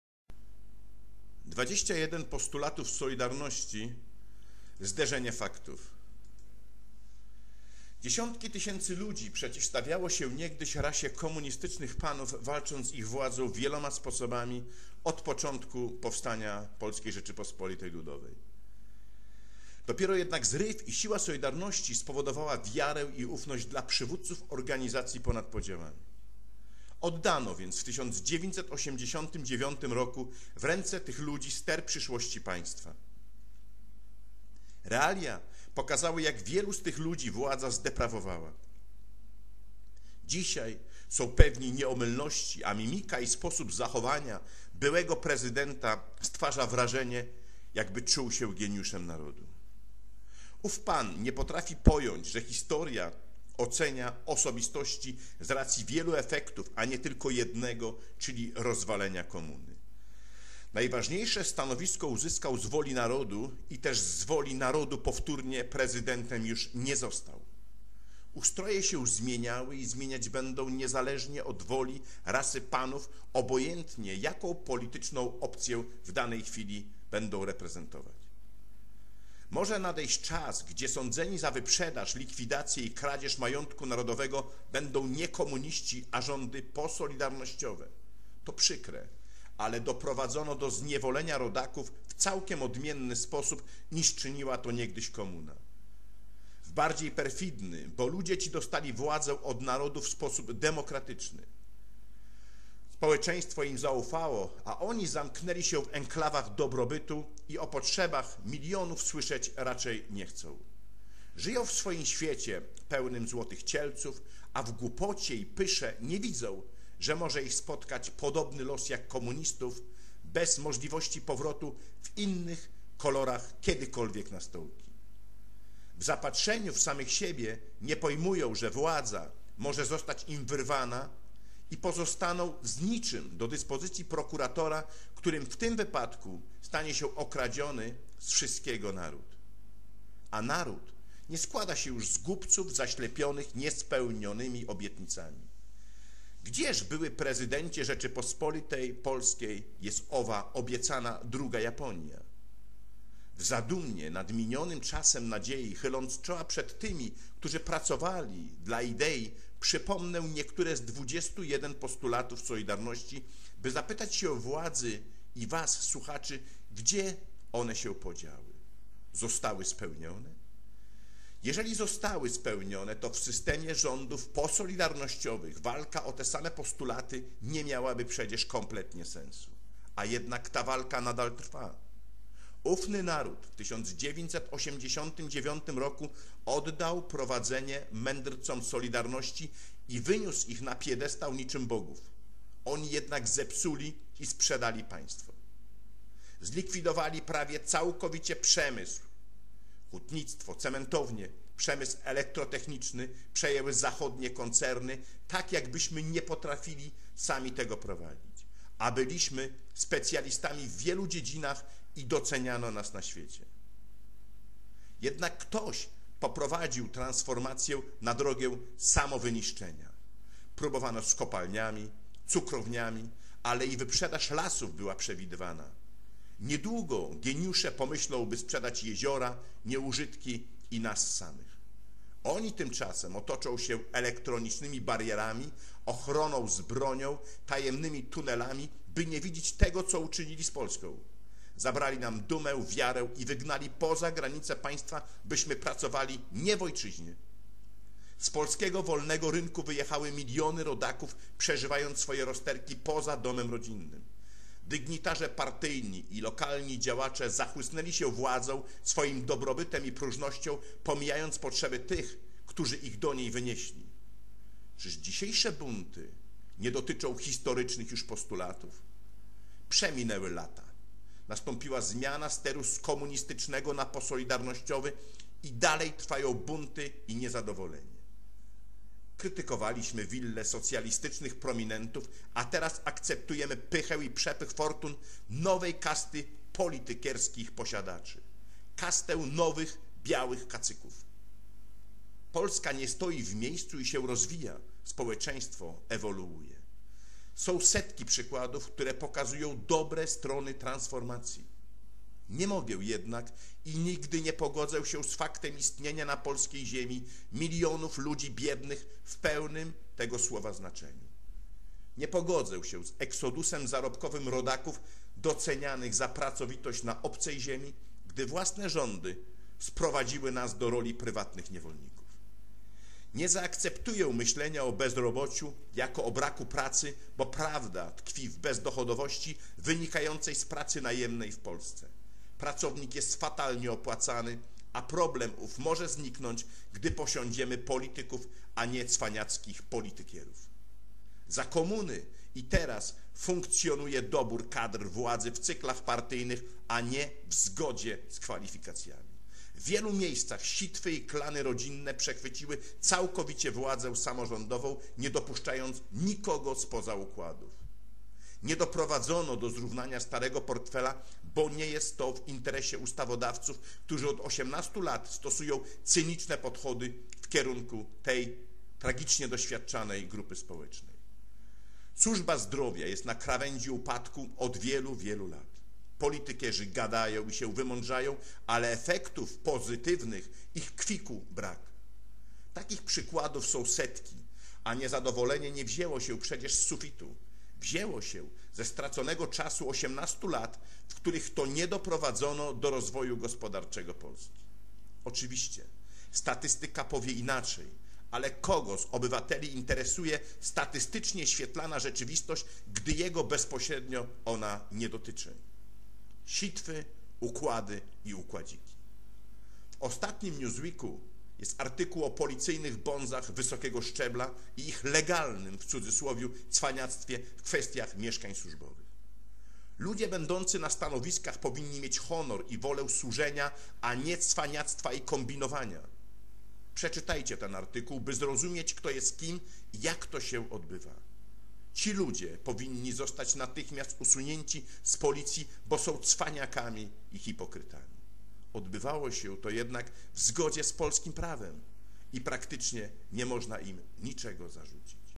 SOLIDARNOŚĆ - ZŁOTY CIELEC WYBRAŃCÓW
Walczono z władzami komunistycznymi od początku powstania PRL.
Dopiero jednak zryw i siła Solidarności spowodowała wiarę i ufność  dla przywódców  organizacji ponad podziałami.
Realia pokazały jak  wielu z nich  władza  zdeprawowała.
Wydaje im się że są nieomylni a mimika i sposób zachowania byłego prezydenta stwarza wrażenie jakby czuł się  geniuszem narodu.
Ów Pan nie potrafi pojąć że historia ocenia osobistości z racji wielu efektów a nie tylko jednego czyli  rozwalenia komuny. Najważniejsze stanowisko uzyskał z woli narodu i też z woli narodu  powtórnie prezydentem nie został .
Ustroje się zmieniały i zmieniać się będą.
Nadchodzi najwyraźniej czas powtórki gdzie sądzeni za wyprzedaż , likwidacje i kradzież majątku narodowego będą nie komuniści a rządy posolidarnościowe.
To przykre ale doprowadzono do zniewolenia rodaków w całkiem odmienny sposób niż czyniła to komuna.
W bardziej perfidny bo ludzie ci dostali władzę od narodu w sposób demokratyczny.
Naród im zaufał a oni zamknęli się w enklawach dobrobytu i o potrzebach milionów słyszeć nie chcą.
Żyją w innym swoim świecie pełnym złotych cielców. W swojej głupocie i pysze nie widzą że czeka ich taki sam koniec jak komuchów .
W swoim szczęściu nie pojmują że władza może zostać im wyrwana i pozostaną z niczym do dyspozycji prokuratora którym w tym wypadku stanie się okradziony z wszystkiego naród.
A naród nie składa się już z głupców zaślepionych niespełnionymi ideałami.
Gdzież były Prezydencie RP jest owa druga Japonia bez rodzimego przemysłu wyprzedanego za bezcen?